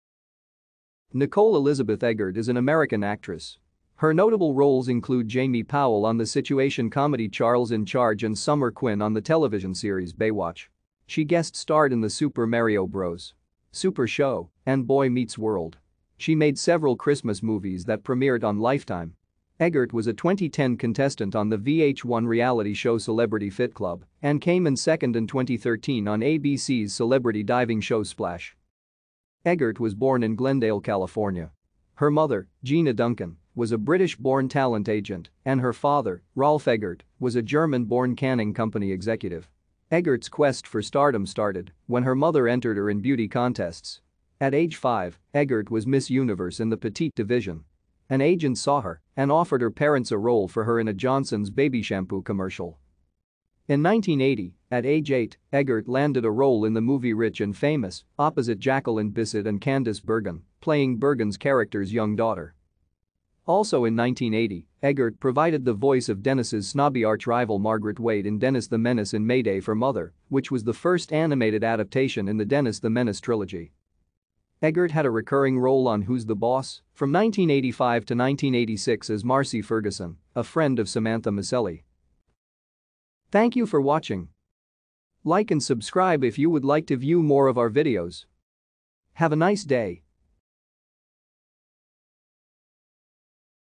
Nicole Eggert
Nicole Elizabeth Eggert  is an American actress. Her notable roles include Jamie Powell on the situation comedy Charles in Charge and Summer Quinn on the   Baywatch. She guest-starred in The Super Mario Bros. Super Show! and Boy Meets World. She made several Christmas s that premiered on Lifetime. Eggert was a 2010 contestant on the VH1 reality  Celebrity Fit Club and came in second in 2013 on ABC's celebrity diving  Splash.